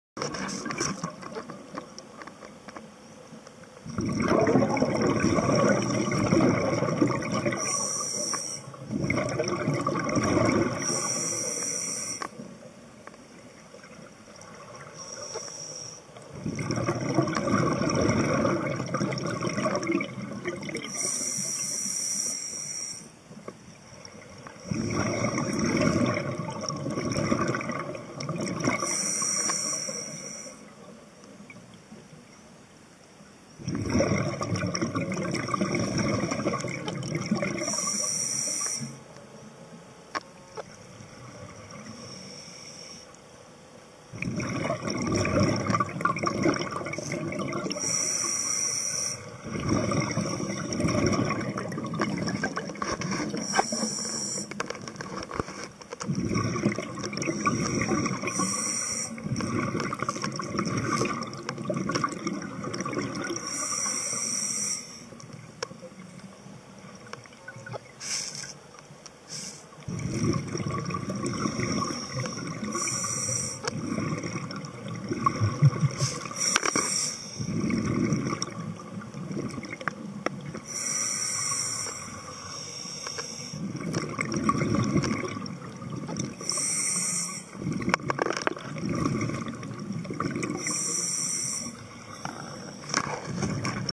Peter meets Woddewick the Wwasse at Wuddy Wocks, St Abbs
Very friendly big Wrasse at Wuddy Rocks. Diving of Billy's boat, Wavedancer out of Eyemouth. Depth around 12m.  7.30pm Friday 14th June 2013. Using my new Olympus TG1 camera with an Olympus housing.